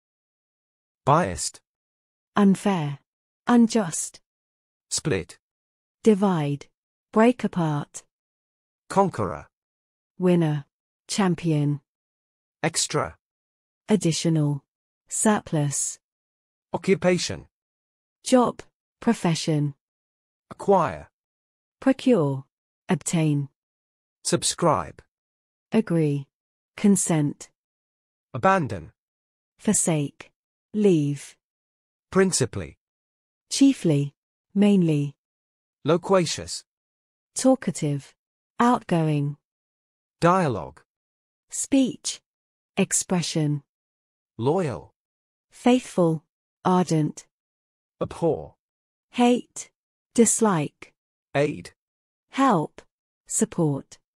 Useful Synonyms in English
In this video, you will learn some useful synonyms in English.
Synonyms and associated words are used to make your text easier to read that’s why it plays an important role in developing your English fluency.
It is highly recommended to use new synonyms as much as possible when you speak and write in English.